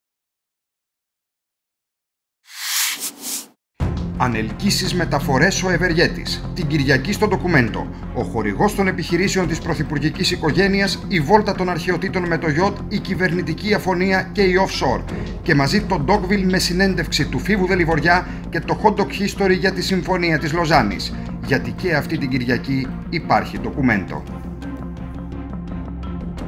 Ανελκύσεις - μεταφορές «Ο ευεργέτης» - Αυτή την Κυριακή στο Documento
Ο χορηγός των επιχειρήσεων της πρωθυπουργικής οικογένειας

Η βόλτα των αρχαιοτήτων με το γιοτ

Η κυβερνητική αφωνία και η offshore

Μαζί το Docville με συνέντευξη του Φοίβου Δεληβοριά και το HotDoc History για τη συμφωνία της Λωζάννης

Γιατί και αυτή την Κυριακή υπάρχει